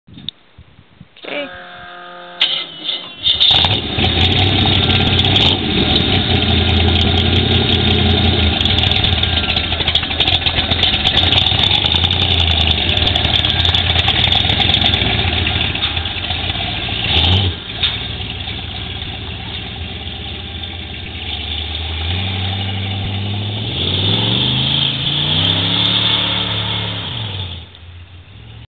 Summit Race mufflers on the ratrod
Summit race mufflers on a 401 AMC with ported manifolds. 2.5"in 2.5" out.  Sorry about the poor video quality.